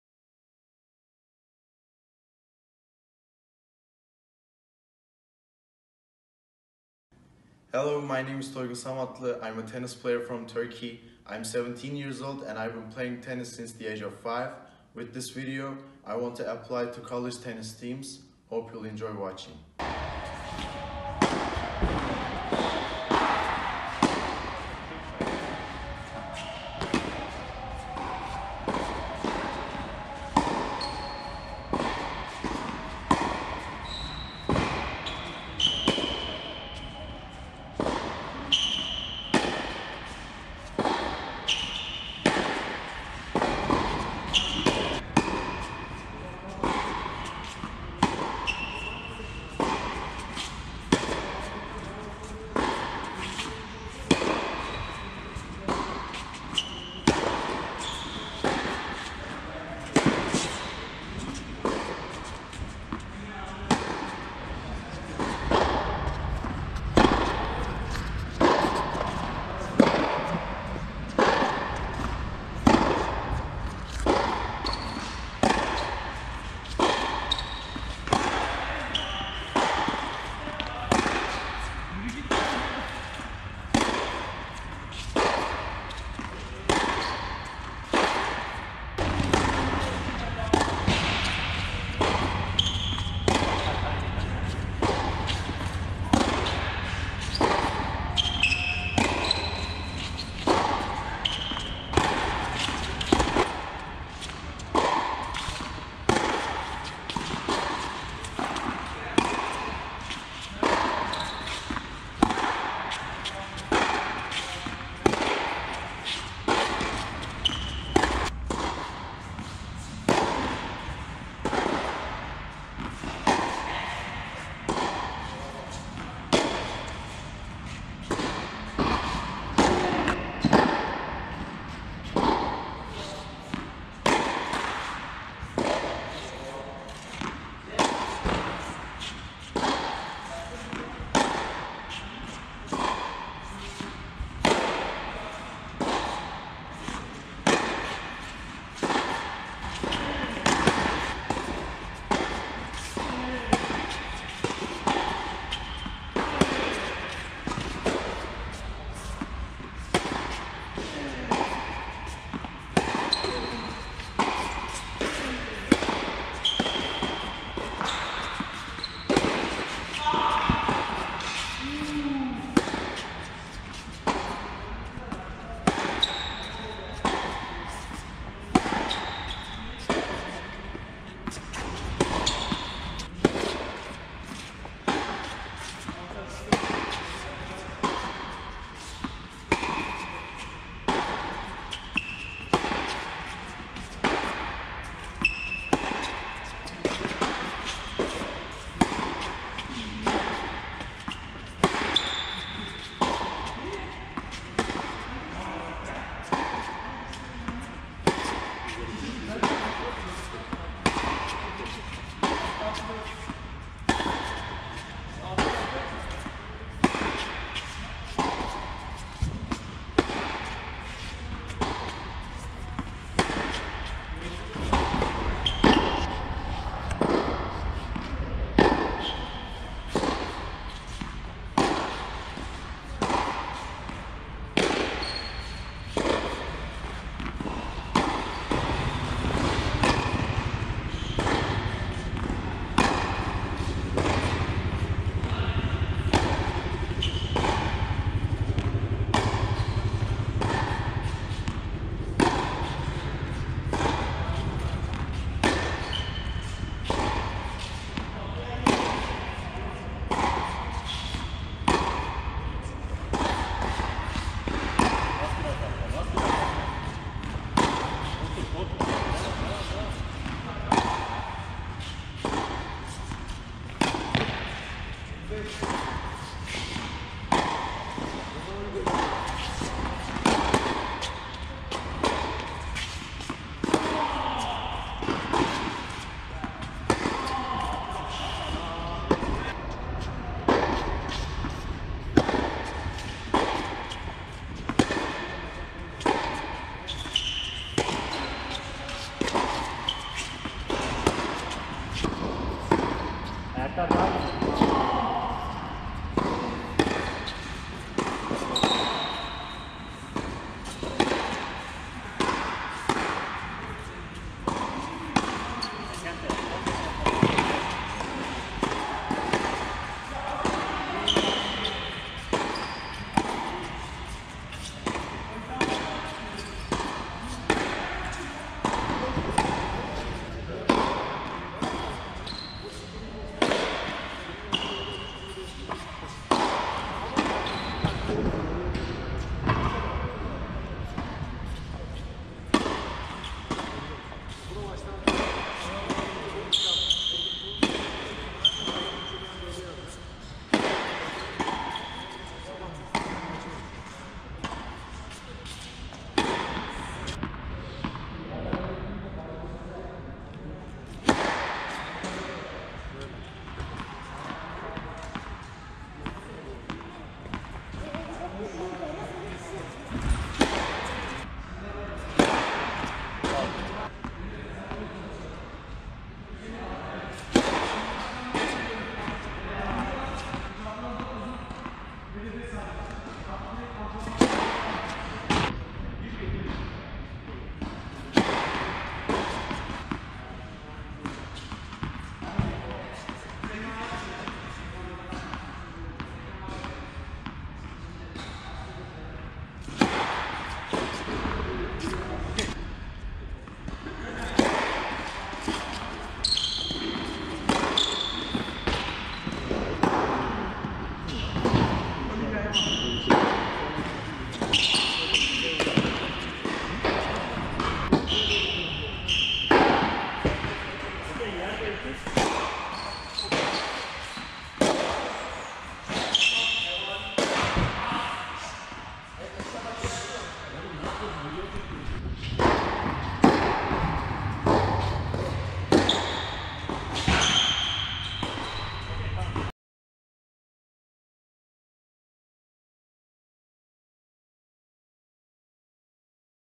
TOYGUN SAMATLI - College Tennis Recruiting Video - Fall 2019
TOYGUN SAMATLI
Country of origin: TURKEY
Date of birth: May 12 2001
School: TED Ankara College
Age Started Tennis: 5
Right-handed